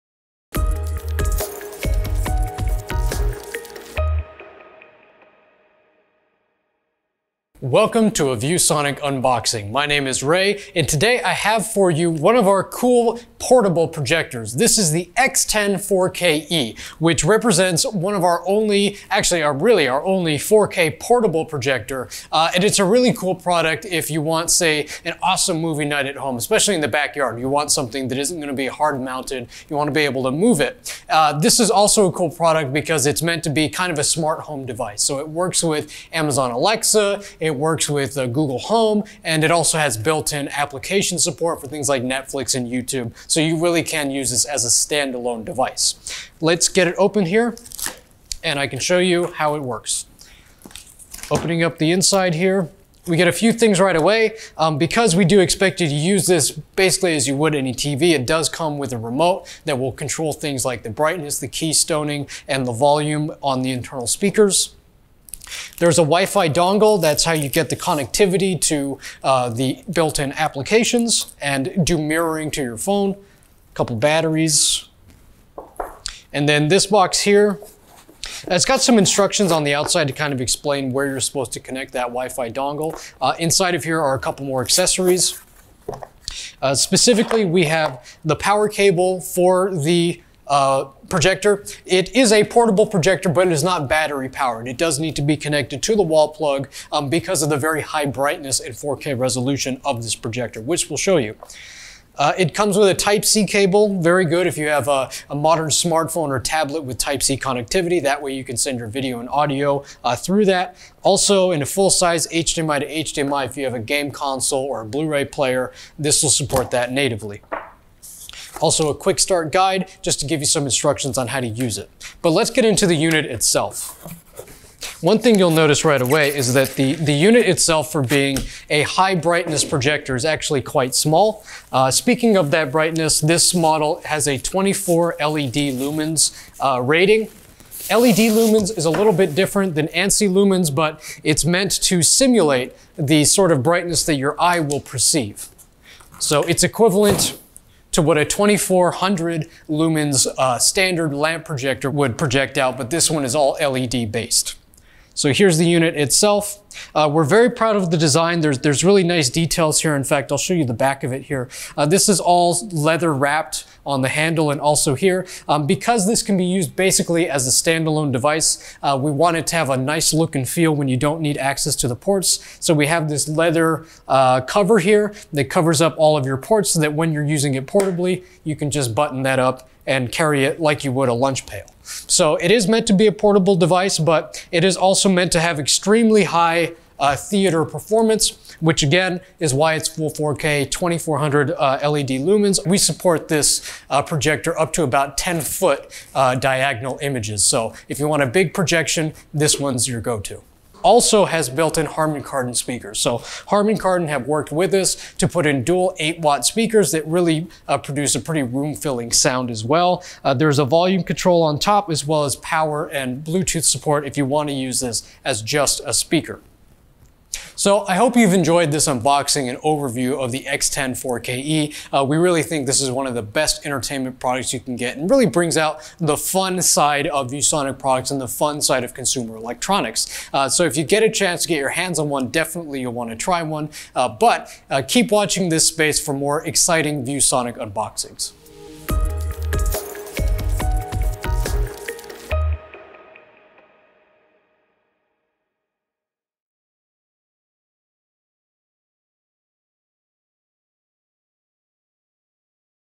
Unboxing the X10-4KE Smart Theater Projector
Combining stunning 4K resolution with smart streaming functionality and a portable design, the ViewSonic X10-4KE projector delivers a cinematic experience from anywhere in your home.